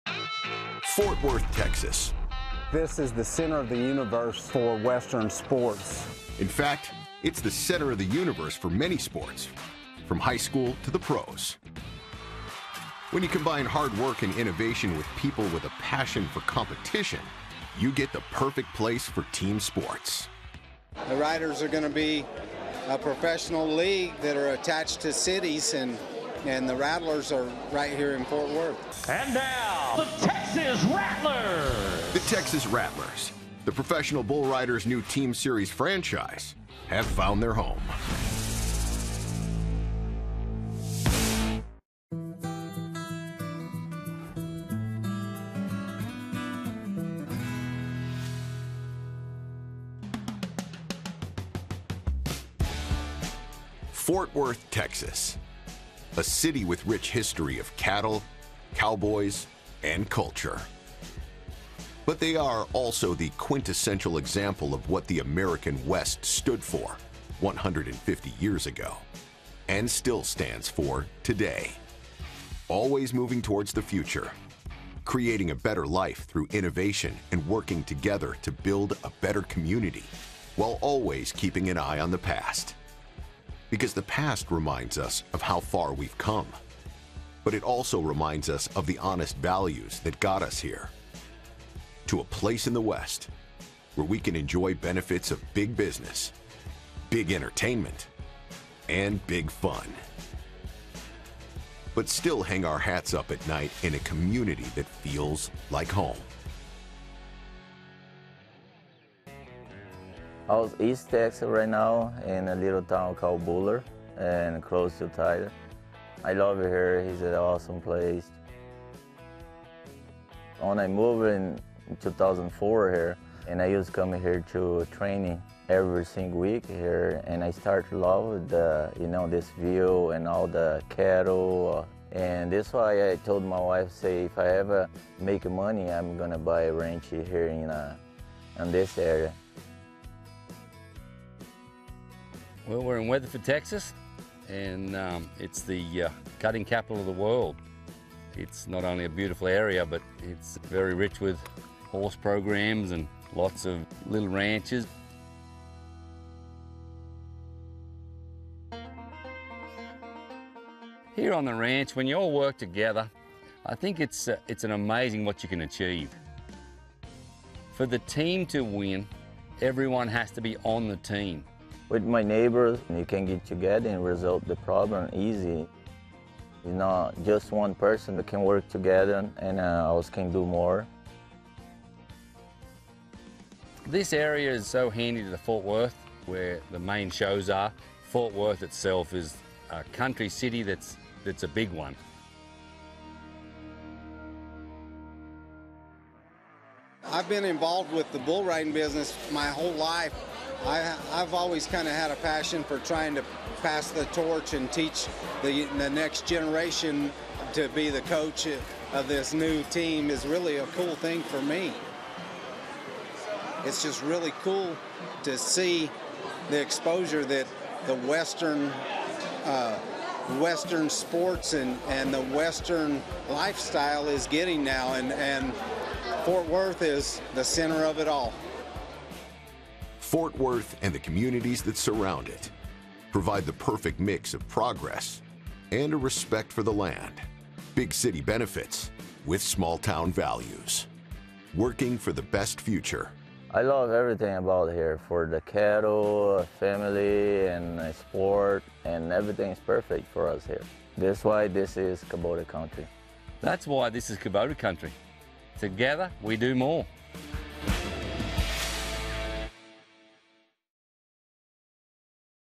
KubotaCountry | PBR Texas Rattlers | Robson Palermo
It doesn't get more western than Cowtown. With a culture built on hard work and camaraderie learn why Fort Worth, Texas is the perfect home for Kubota equipment and the PBR Texas Rattlers.  Learn how Robson Palermo uses his Kubota to get the job done day in and day out.